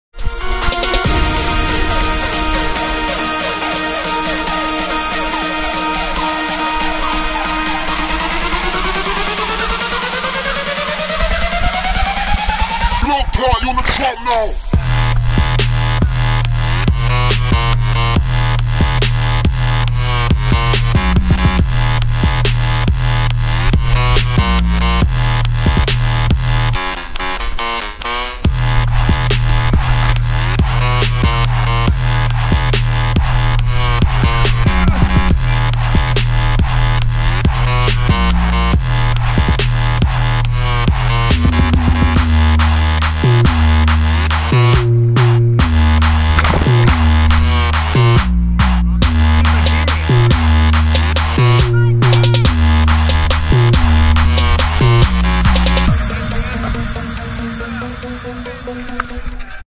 Spartan Race in One Minute
A compilation of the highlights from my team's Spartan Race, as seen through Garrett's GoPro.